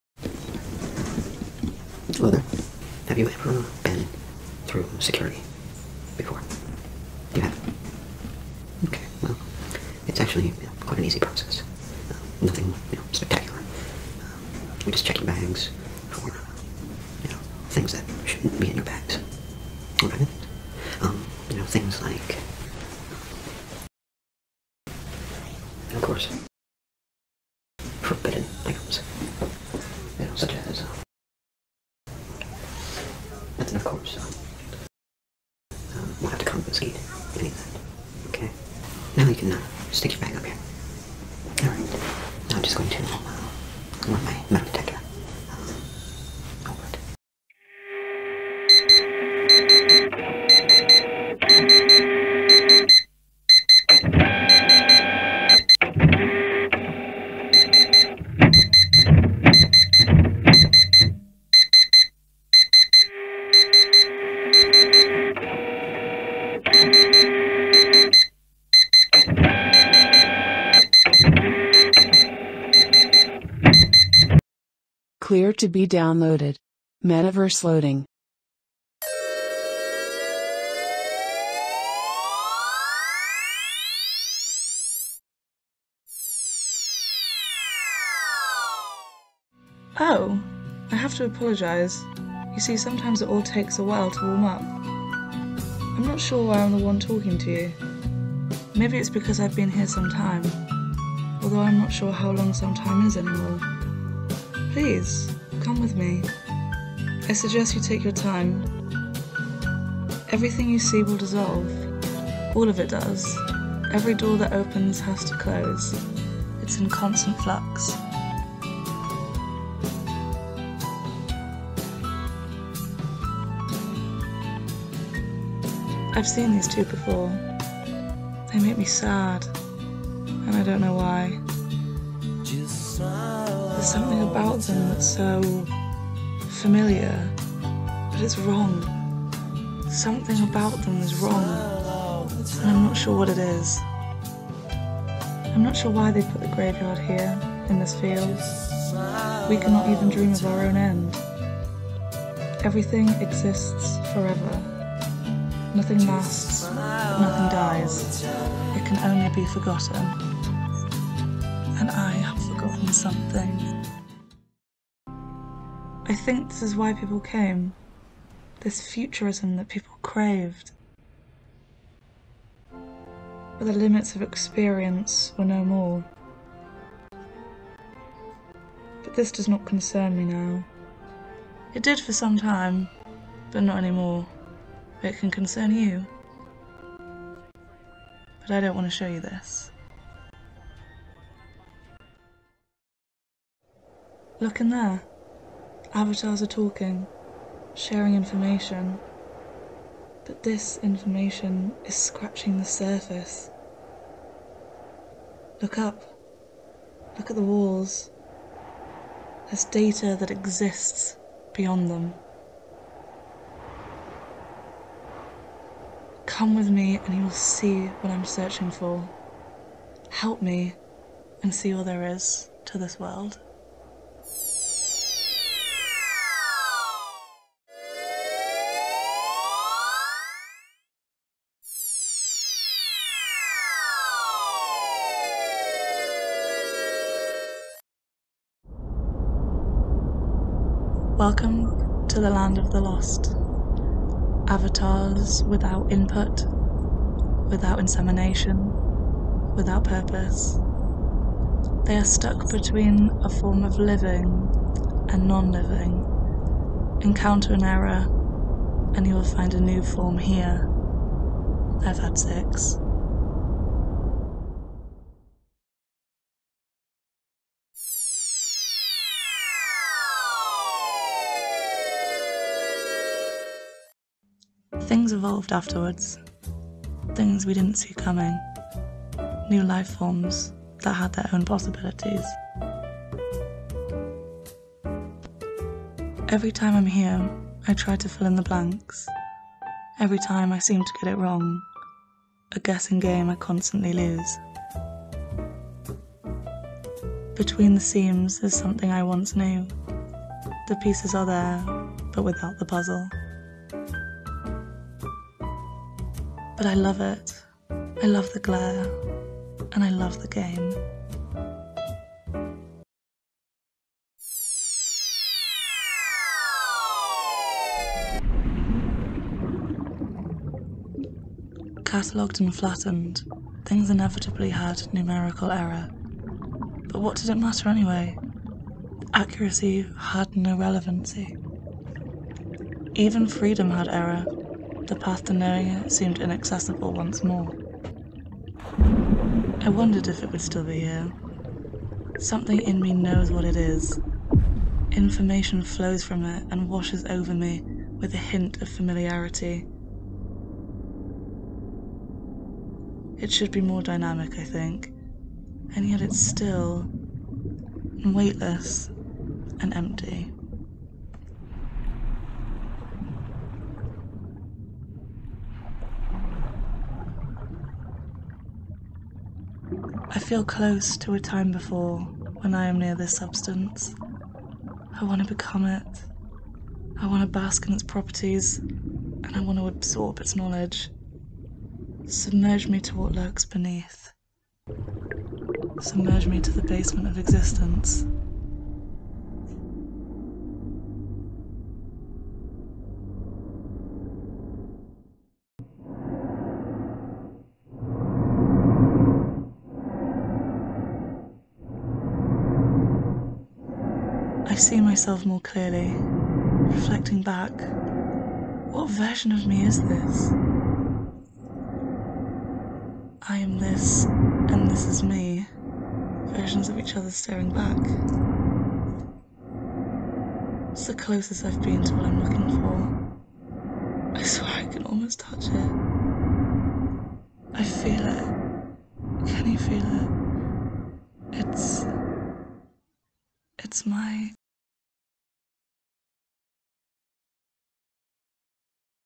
Finding Flesh
A narrative through the metaverse to rediscover a physical body made of flesh.